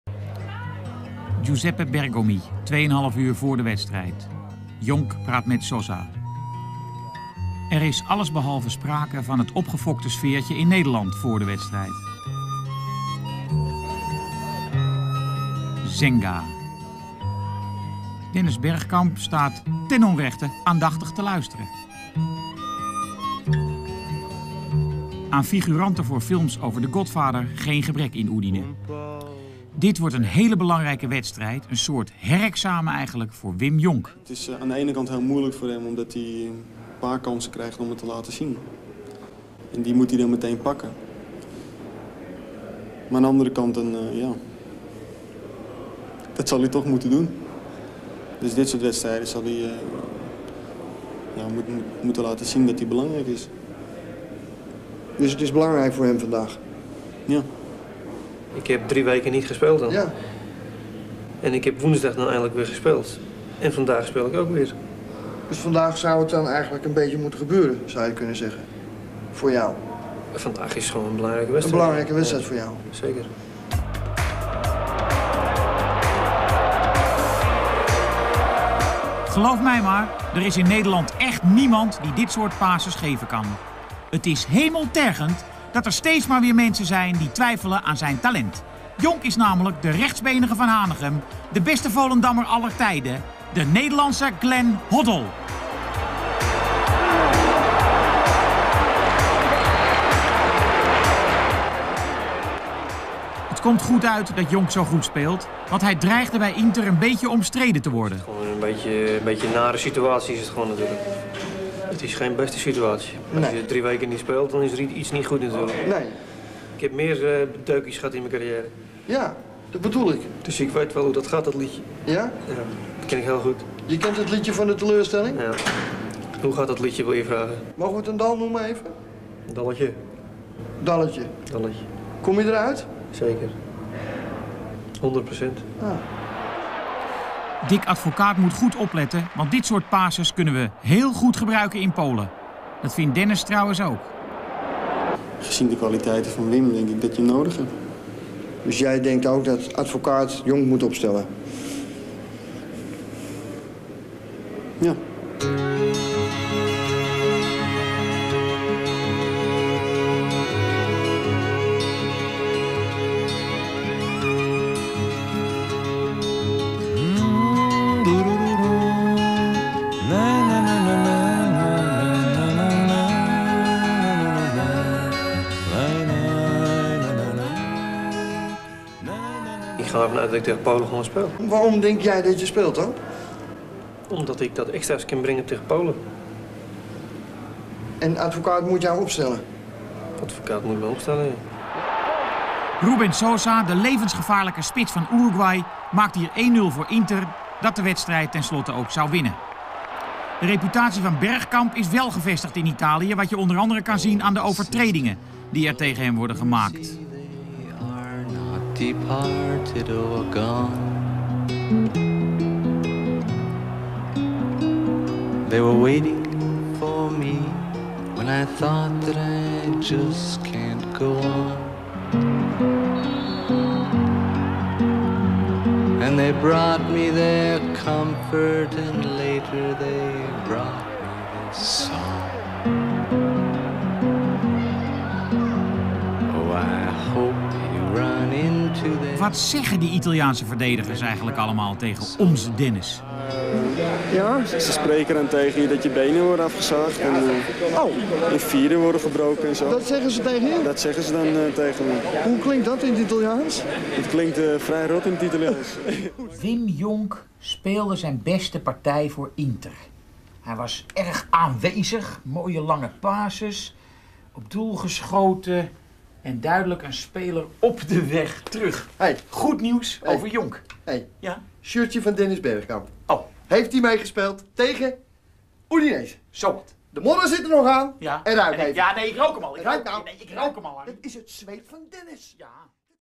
Harry in Italië bij Bergkamp en Jonk!
Harry reist af naar Italie en praat vlak voor de wedstrijd van Inter Milaan uit tegen Udinese nog even met Dennis Bergkamp en Wim Jonk!!